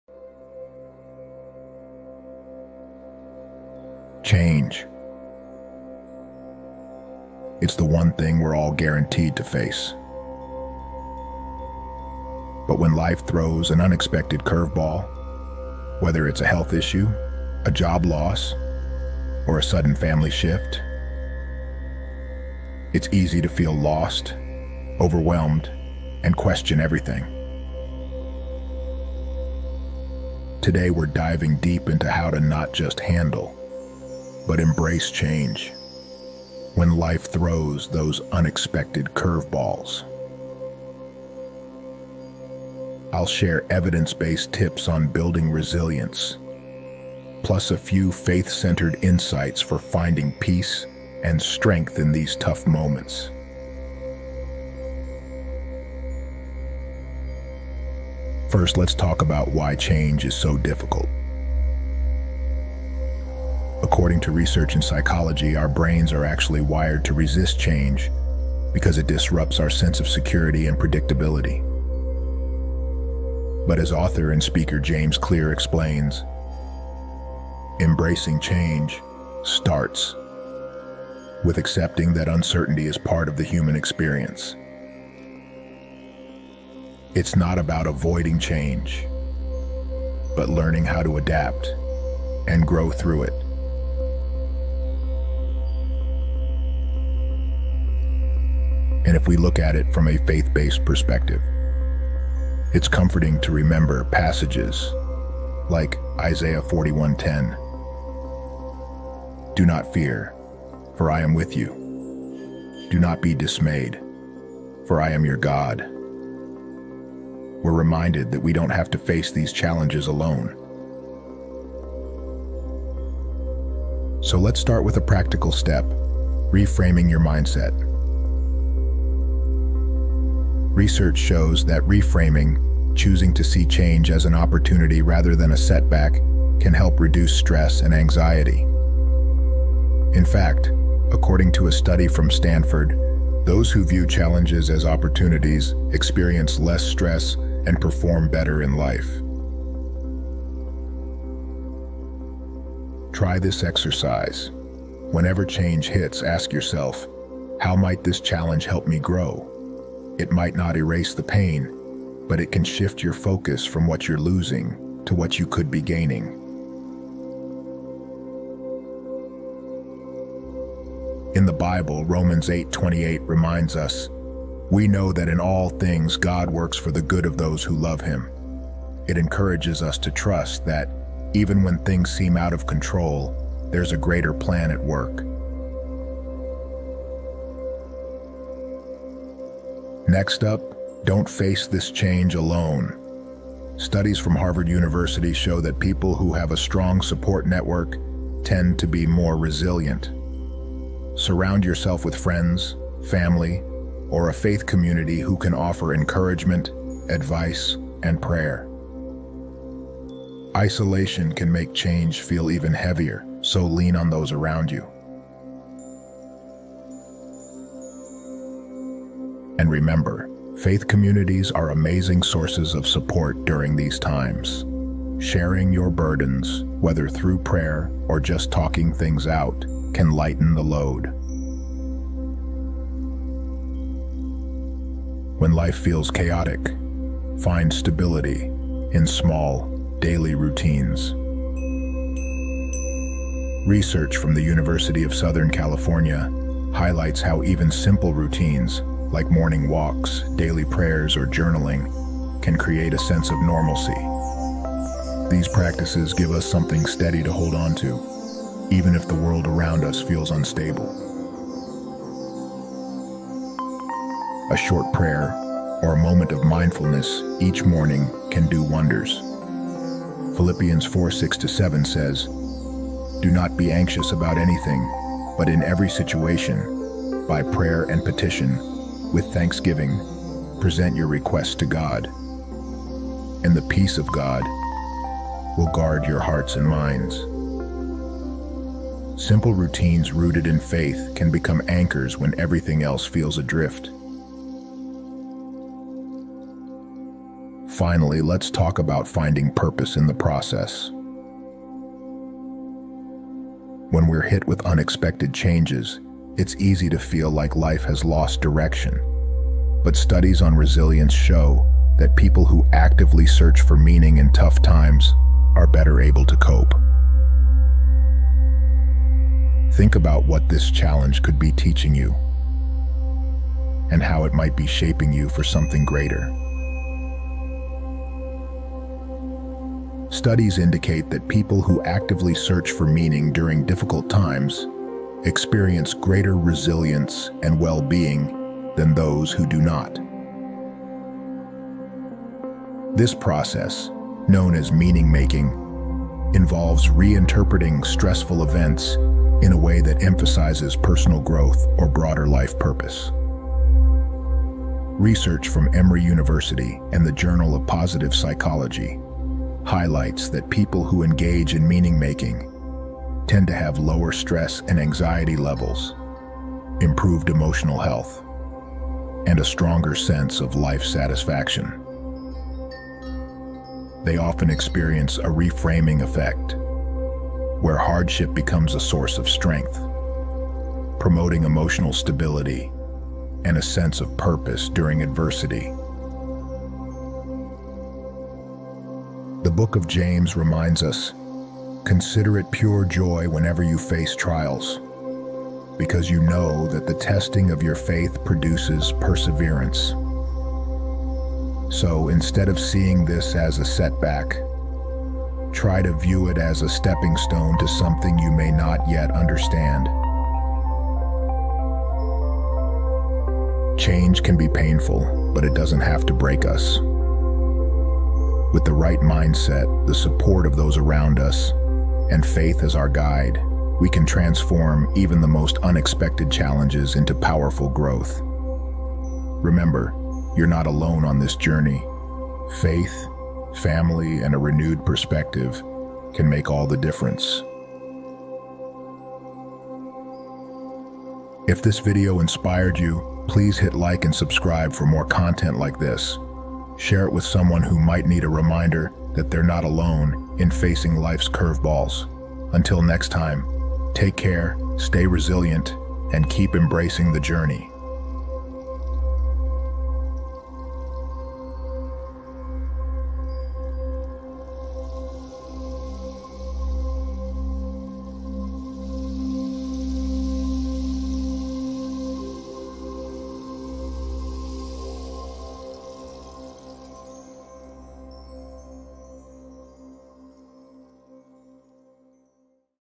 Embrace Life’s Curveballs: Proven Strategies to Thrive Through Unexpected Change!
When life throws you an unexpected curveball—a health issue, job loss, or sudden family change—it’s natural to feel overwhelmed. In this video, we’ll show you how to face life’s toughest challenges head-on with resilience, faith, and purpose.

This video covers:

🔶Why Change Feels So Hard: Discover the science behind why our brains resist change and how to break free from this pattern.

🔶Reframe Your Mindset: Learn how to turn obstacles into growth opportunities with tips inspired by Romans 8:28.

🔶Building a Support Network: Understand the power of faith communities, family, and friends in staying strong through life’s shifts.

🔶Daily Routines for Stability: Create simple, grounding routines—backed by USC research and Philippians 4:6-7—to regain peace.

🔶Finding Purpose in Challenges: Discover how trials can shape us for the better, with wisdom from the Book of James.

Join us for a journey that blends psychology-backed strategies and faith-centered insights to help you navigate life’s unexpected twists. You don’t have to face these changes alone—find the strength to turn curveballs into stepping stones.